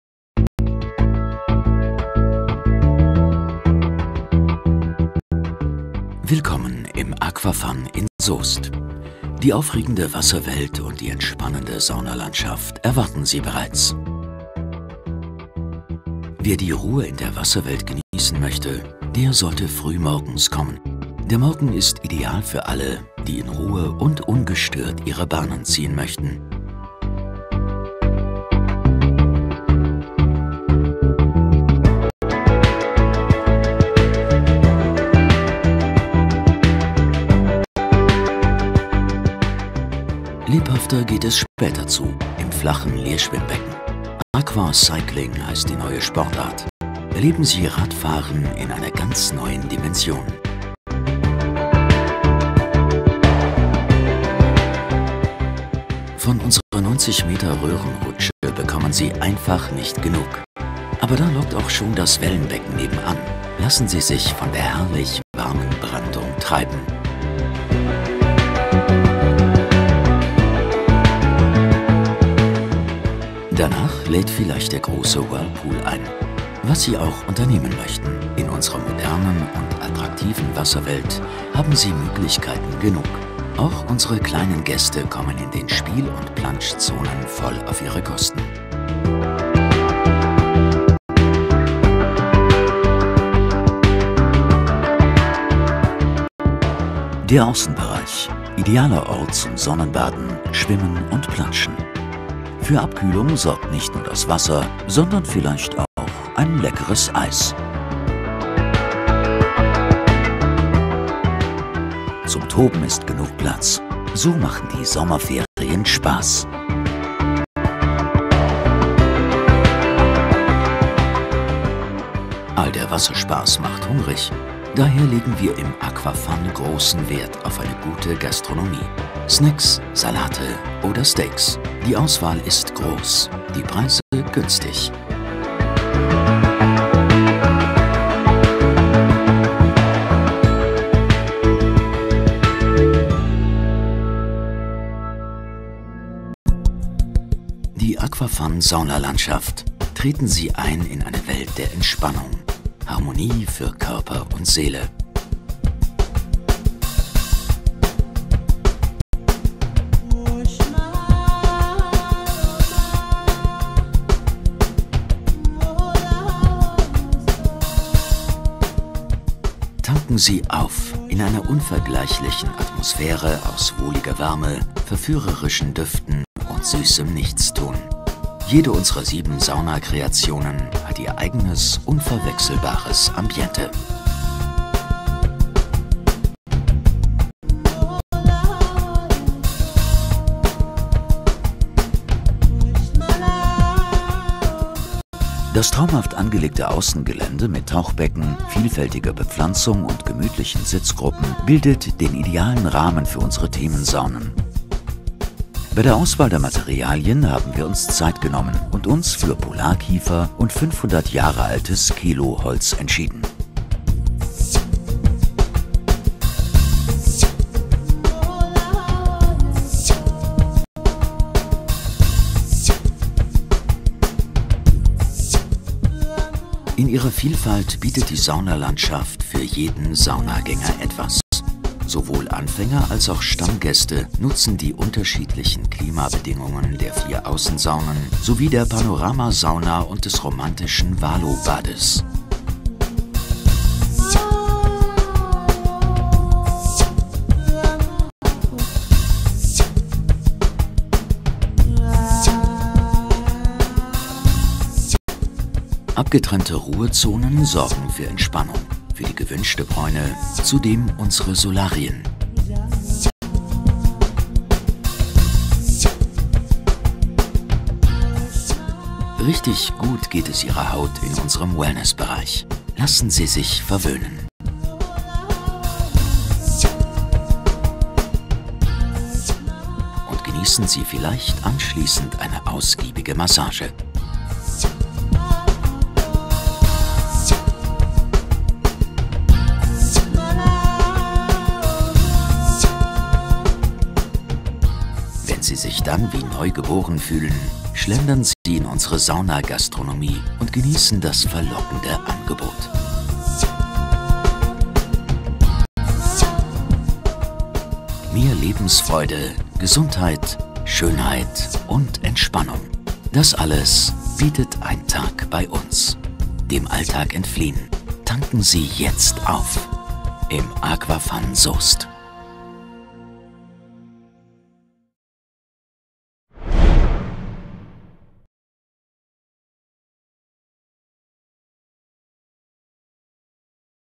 Aquafun Soest
AquaFun Soest - Das Bad für die ganze Familie!
Das Bad lässt keine Wünsche offen und bietet ein umfassendes Angebot für die ganze Familie: Sport- und Freizeitbad, Saunalandschaft, Wellnessparadies und nicht zuletzt das Freibad mit großer Liegewiese. Der Film zeigt Impressionen, um sich auf einen Besuch im Aquafun vorzubereiten.